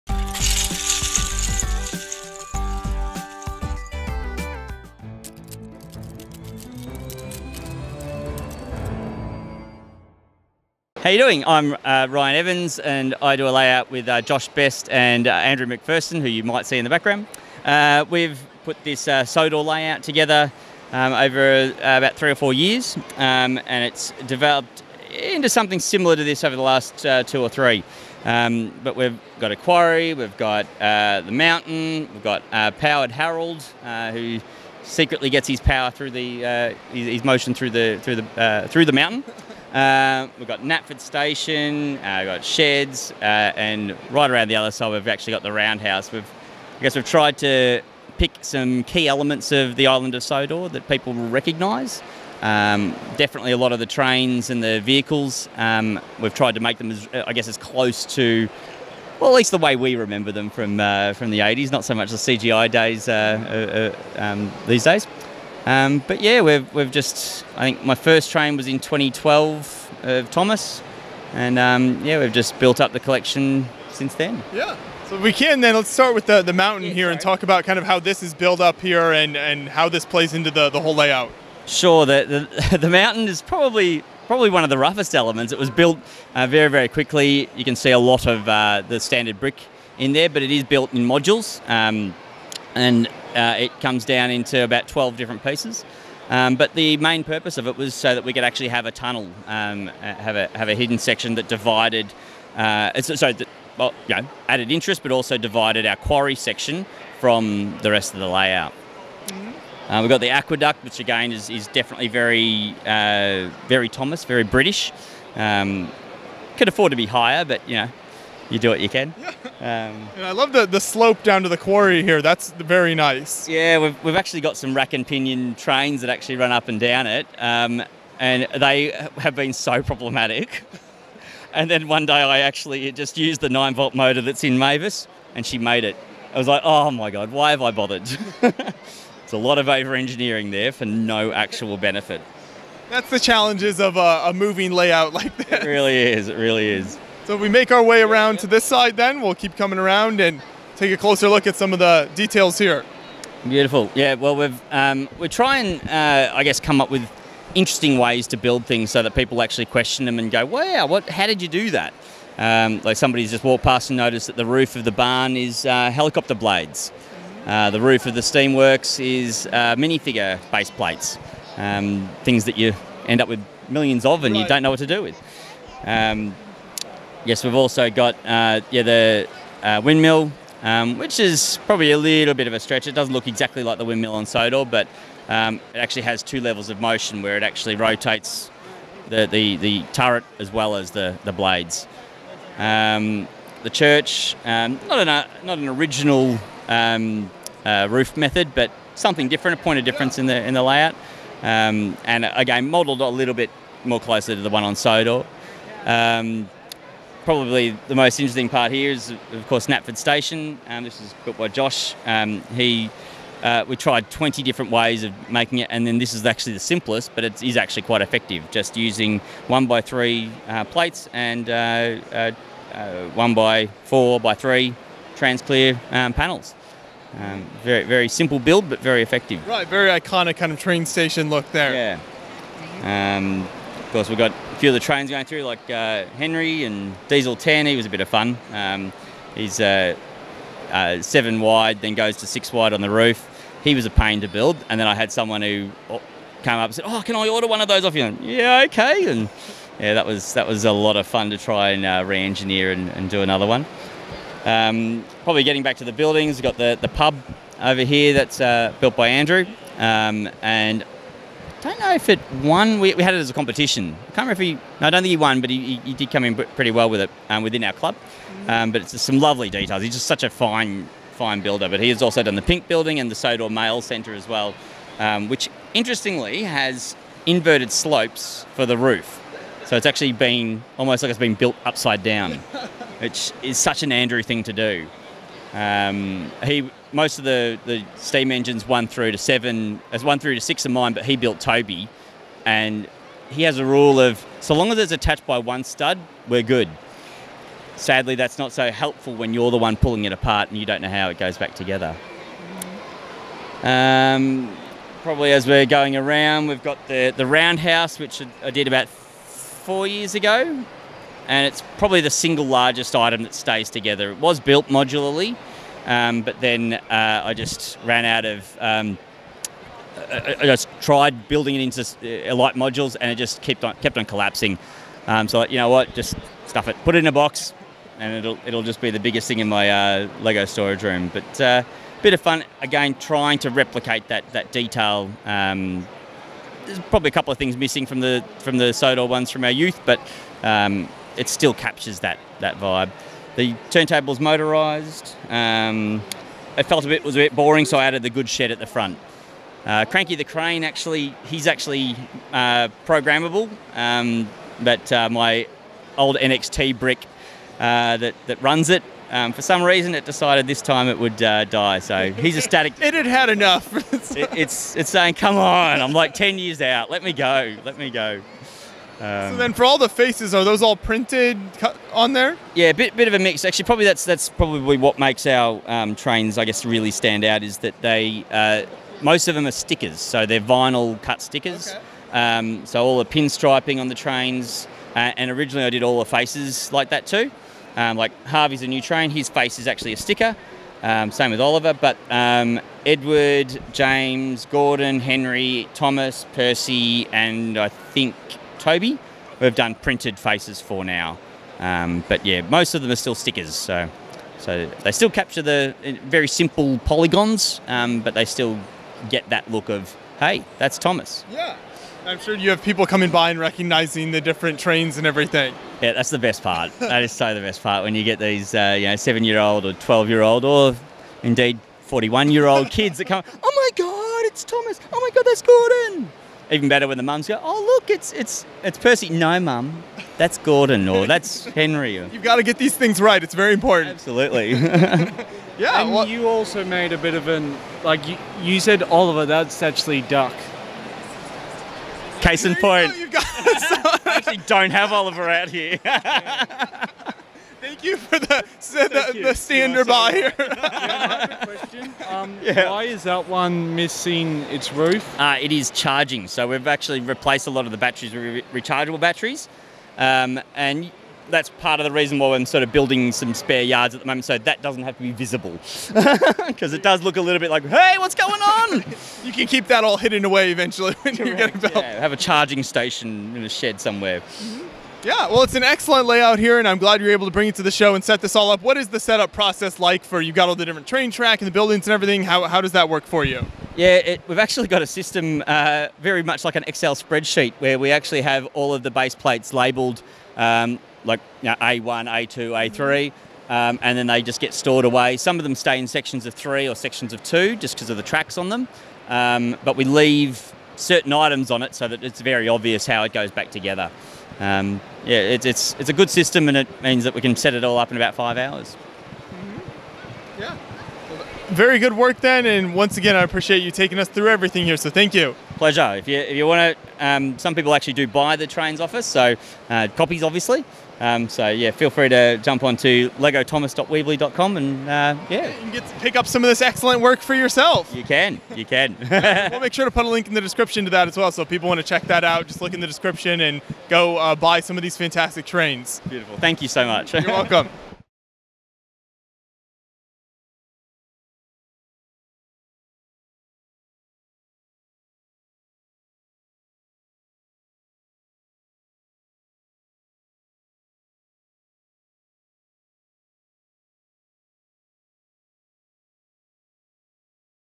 LEGO Island of Sodor Custom Train City | Brickvention 2019
Beyond the Brick's Joshua Hanlon talks with Ryan Evans about the LEGO Island of Sodor train display at Brickvention 2019. For more information or to purchase a model,